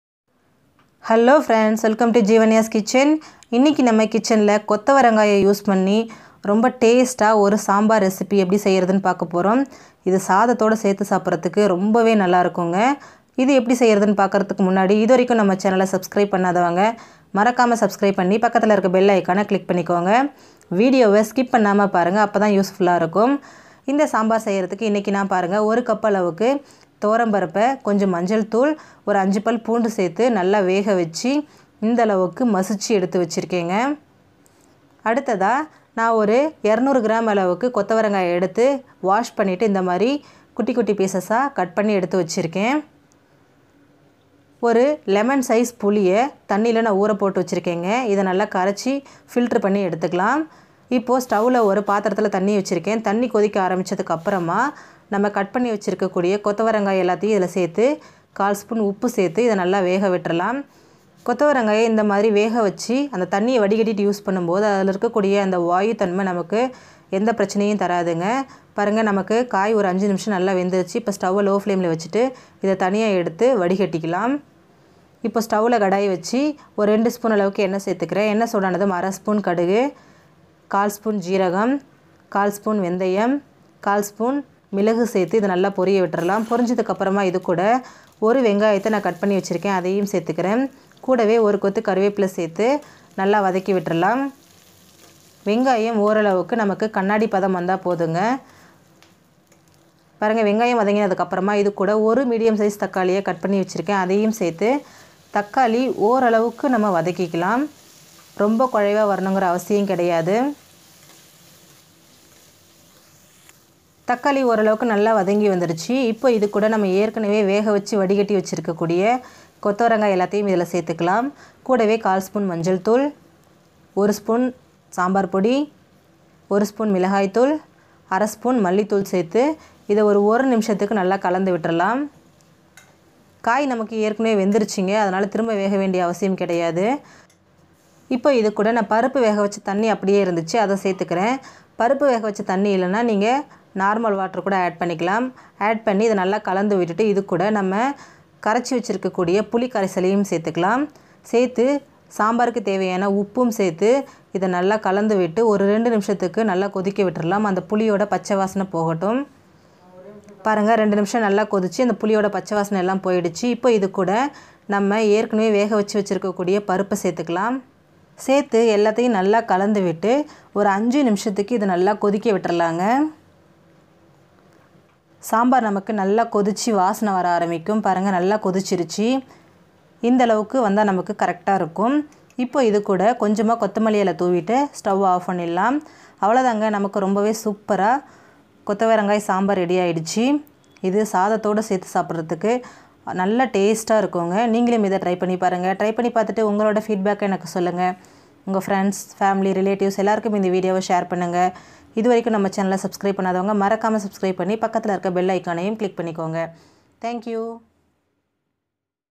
கொத்தவரங்காய் சாம்பார்/ kothavarangai sambar recipe/ Cluster beans recipe in Tamil
In this video we will see how to make cluster sambar in Tamil . This sambar is a wonderful combination for rice.